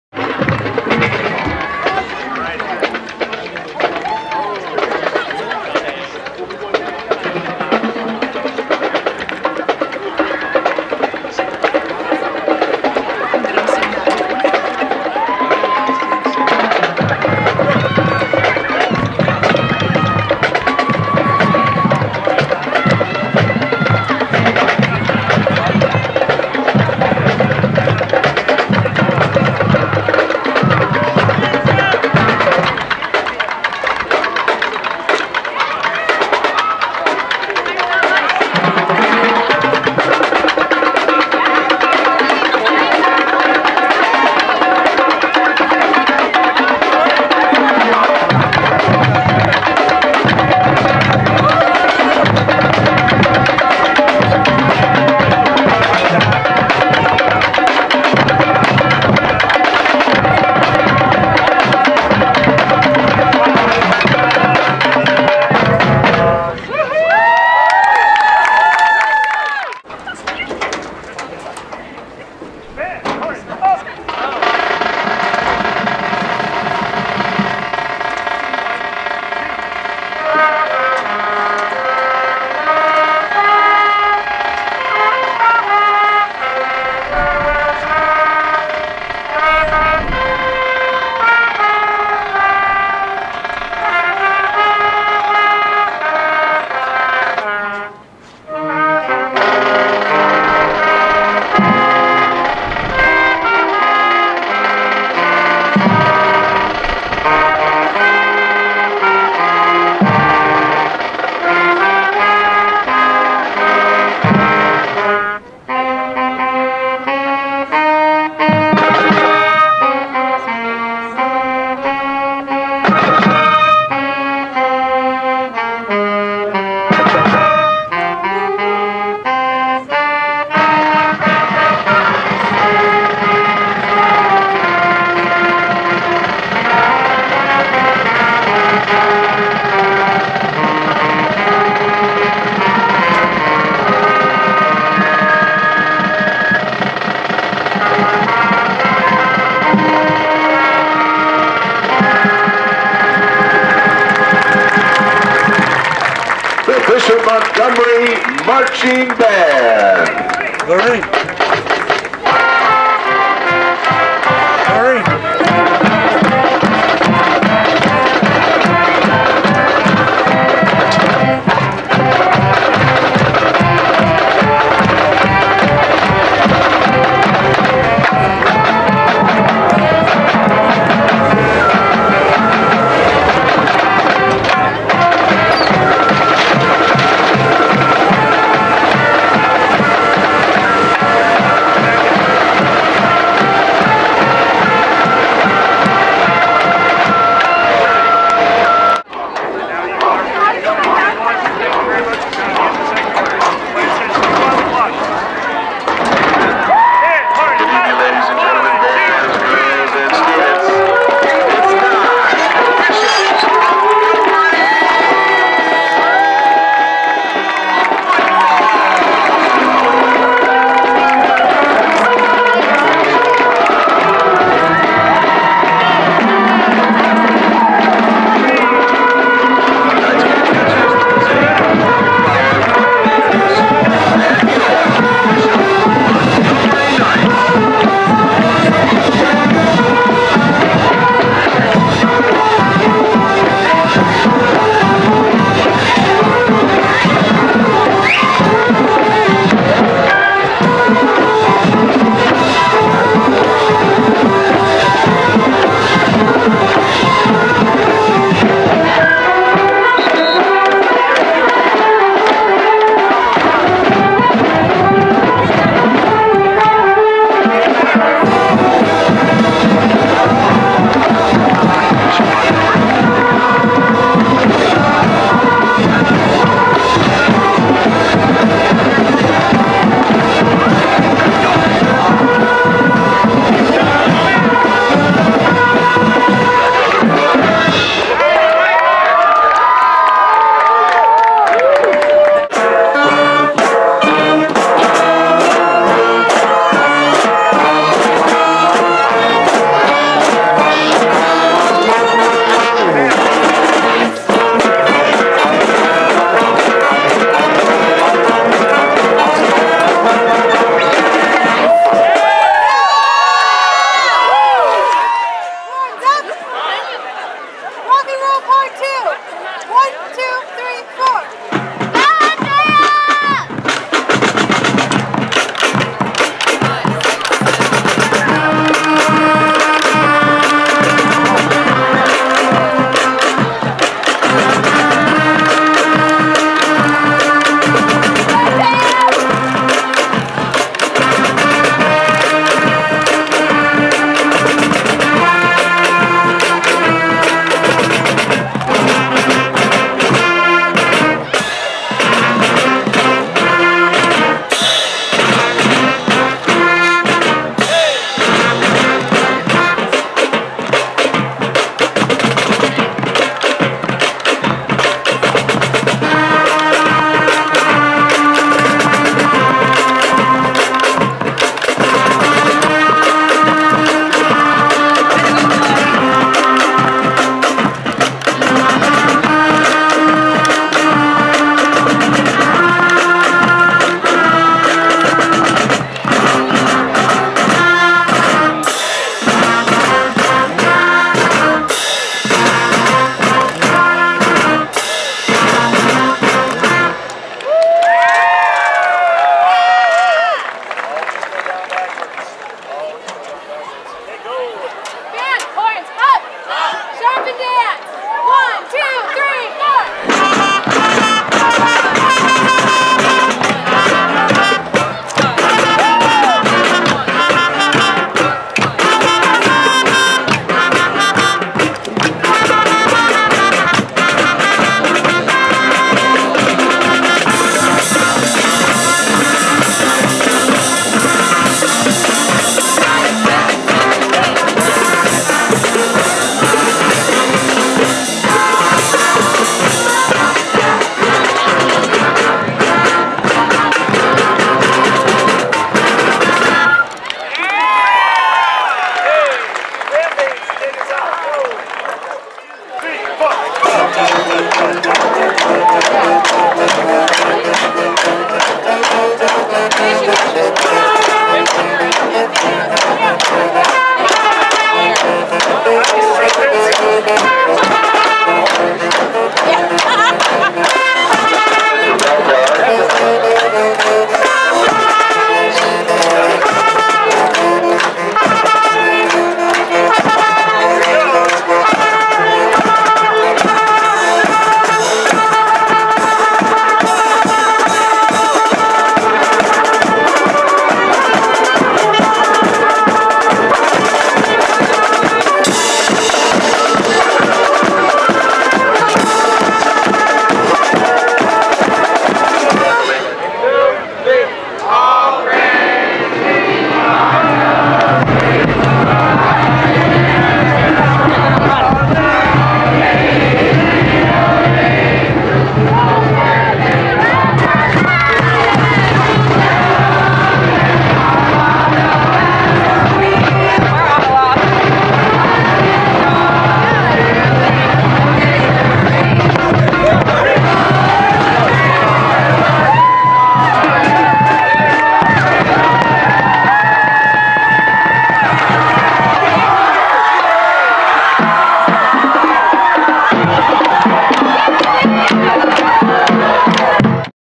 BMHS Knight Band and Guard at Home Game Sept.16, 2016
The Bishop Montgomery Knight Band and Guard performs in a home game for the first time this year. There are many new band members and to many of them, this will be their first time in front of a large audience. Obviously, it's pretty thrilling for parents to see their kids on the field too. Go Knights!